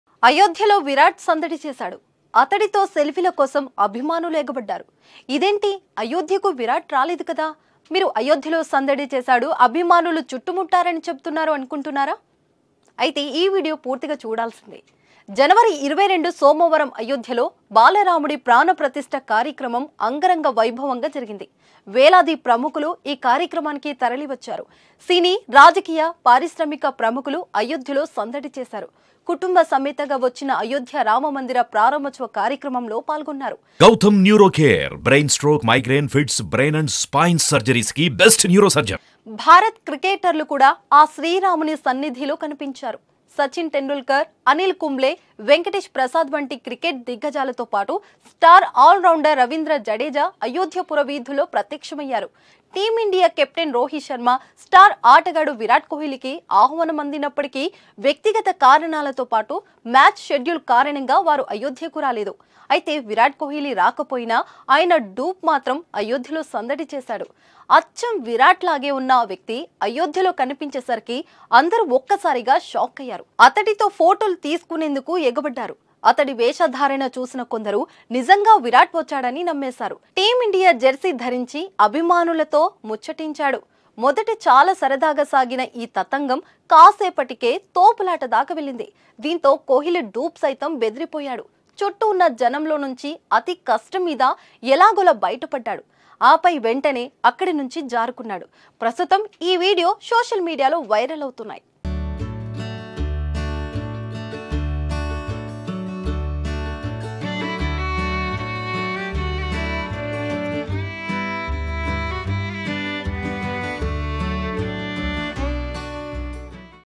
అయోధ్య లో సందడి చేసిన విరాట్ సరదా పడ్డ భక్తులు | Virat Kohli Ram Mandir
Watch ►► అయోధ్య లో సందడి చేసిన విరాట్ సరదా పడ్డ భక్తులు | Virat Kohli Ram Mandir |  @SumanTVSiricilla